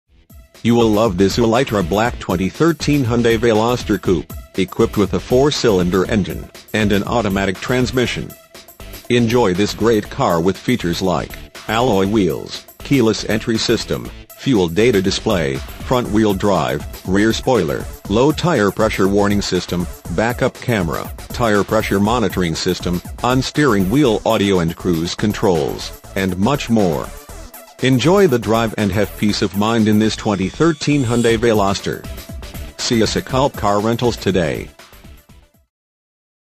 2013 Hyundai Veloster Kulp Car Rentals Gilbertsville, PA 19525
Hyundai Veloster Imagine driving this Ulightra Black Hyundai Veloster Coupe, equipped with a 4 Cylinder engine and an automatic transmission. Enjoy this great car with features like Verify Options Before Purchase, Alloy Wheels, Keyless Entry System, Fuel Data Display, Front Wheel Drive, Rear Spoiler, Low Tire Pressure Warning System, Back Up Camera, Tire Pressure Monitoring System, on steering wheel audio and cruise controls, and much more. Enjoy the drive and have peace of mind in this Hyundai Veloster. See us at Kulp Car Rentals today!
MILES:    843
VIN:KMHTC6AD1DU167227

1828 Swamp Pike
Gilbertsville, Pennsylvania 19525